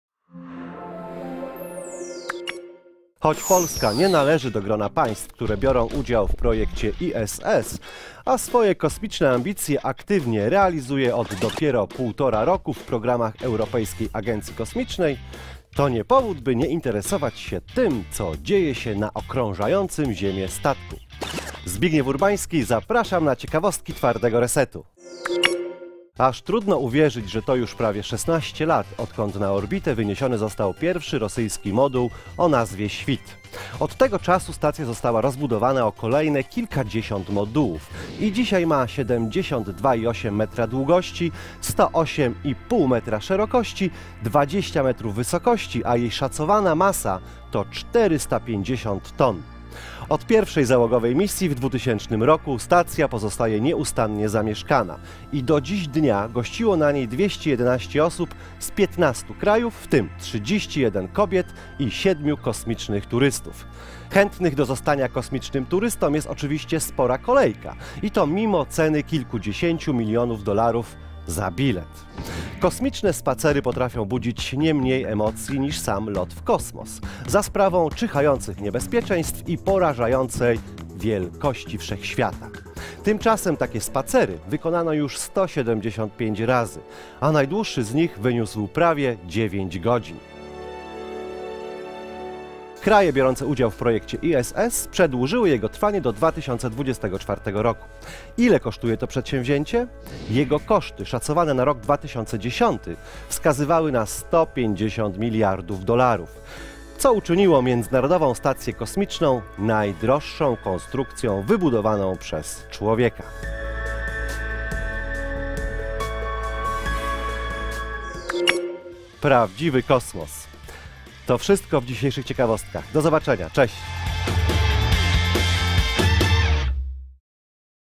Tajemnice Międzynarodowej Stacji Kosmicznej - Twardy Reset
W tym odcinku Twardego Resetu przyjrzymy się nieco bliżej najdroższej konstrukcji kiedykolwiek zbudowanej przez człowieka - Międzynarodowej Stacji Kosmicznej.